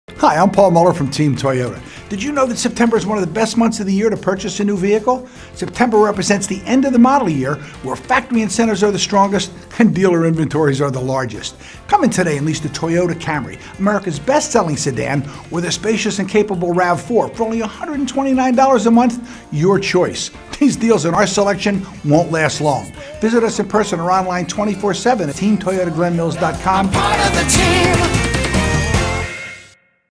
Team Toyota Of Glen Mills - September Sales Event
The September Sales Event is Going on Now at Team Toyota of Glen Mills, and its the sales event you don’t want to miss.  Visit TeamToyotaGlenmillscom For all the details!